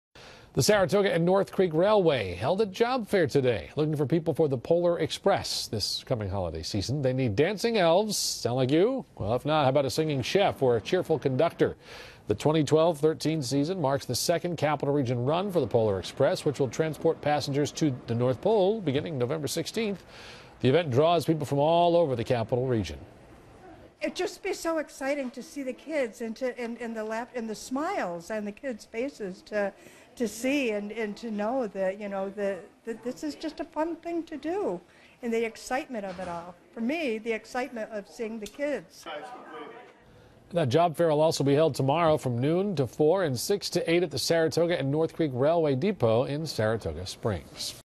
Polar Express hiring for the season
SARATOGA SPRINGS -- The Saratoga and North Creek Railway held a job fair Friday looking for people to work for the Polar Express this coming holiday season.They are seeking dancing elves, singing chefs, and cheerful conductors.    The 2012-13 season marks the second Capital Region run for the Polar Express, which will transport passengers to the North Pole beginning November 16.The event drew people from all over the capital region.The job fair will also be held Saturday from noon to 4 p.m. and 6 to 8 p.m. at the Saratoga